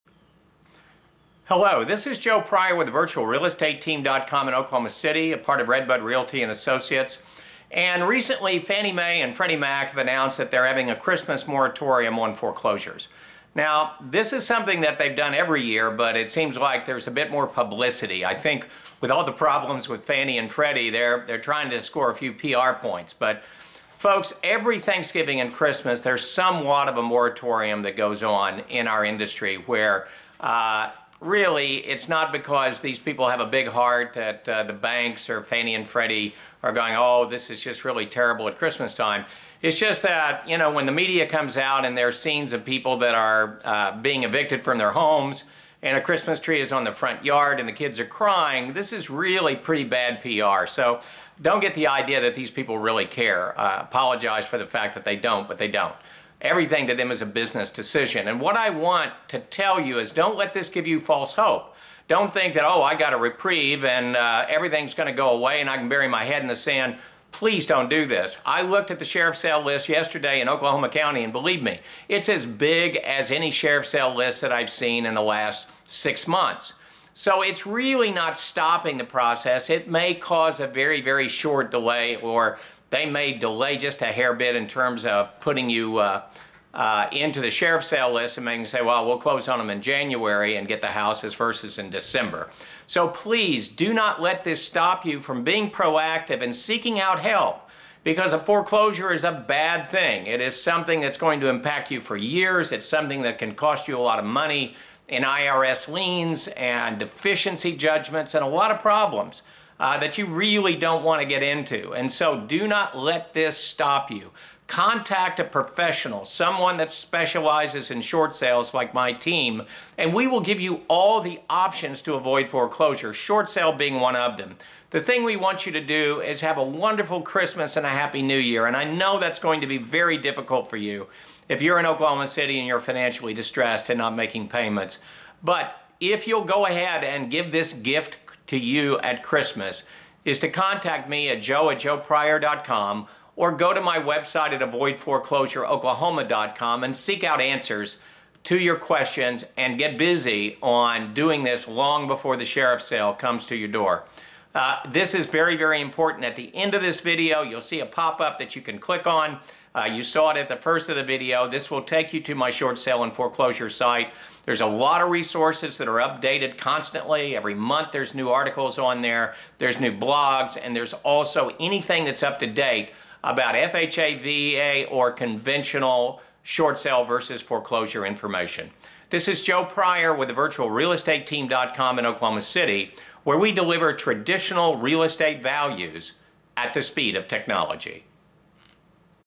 Fannie Mae and Freddie Mac Foreclosure Moritorium
In you are a financially distressed homeowner in Oklahoma City Oklahoma or anywhere just becasue these mortgage giants called for a temporary halt to evcitions doesn't mean you are safe. This is just a short blip on the way to foreclosure. They do this every year normally without publicity not because the love you, it is because they don't want it to hit the news during the holiday.